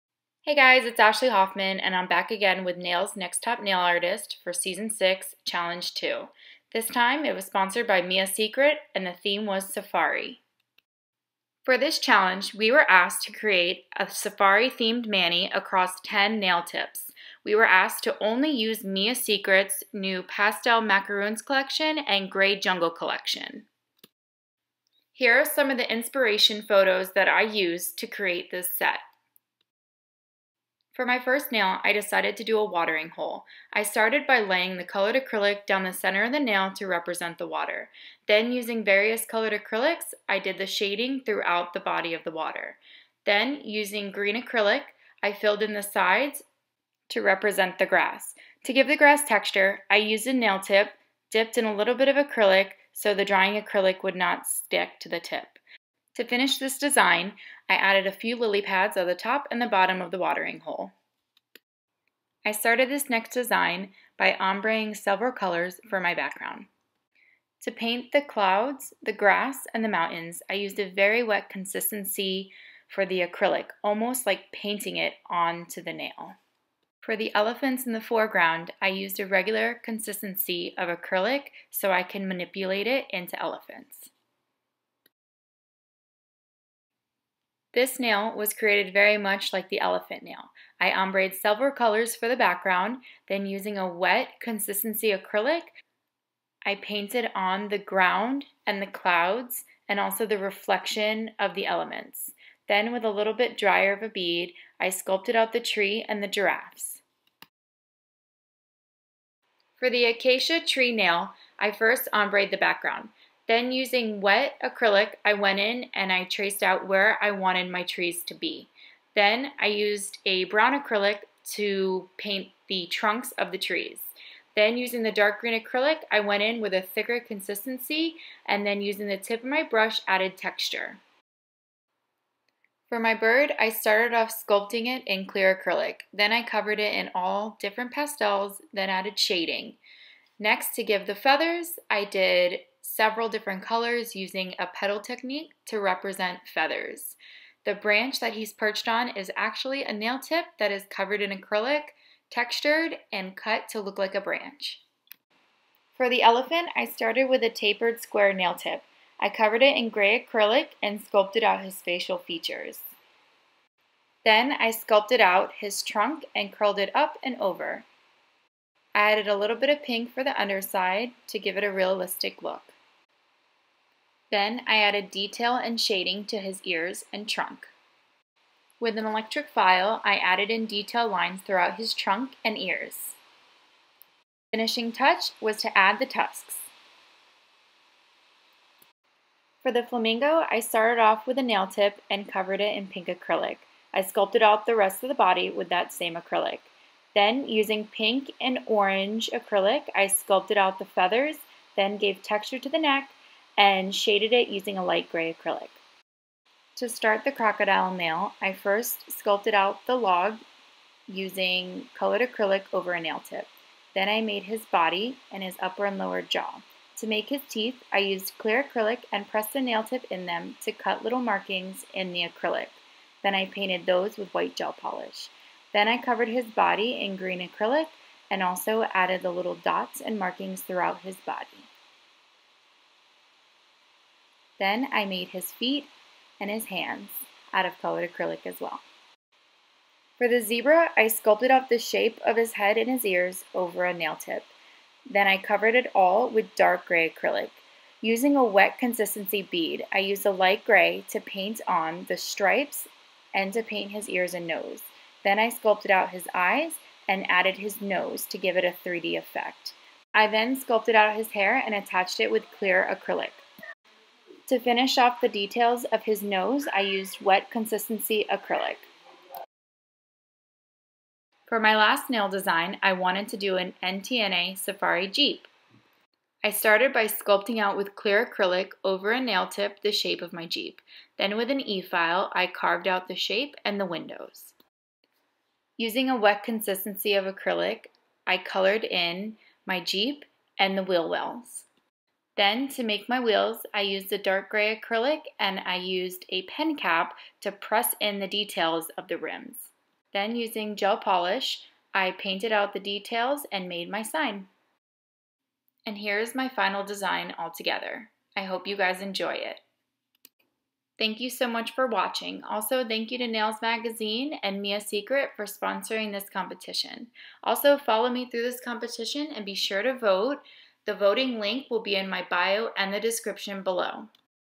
NTNA Season 6 Challenge 2 "Safari" sponsored by Mia Secret
Ashley Hoffman's Entry Video for NTNA Season 6 Competition 2 Sponsored by Mia Secret.

Voting starts Monday1/28 until Thursday 1/31. Be sure to vote!!!

Follow me through this competition on Instagram @The_Nail_Studio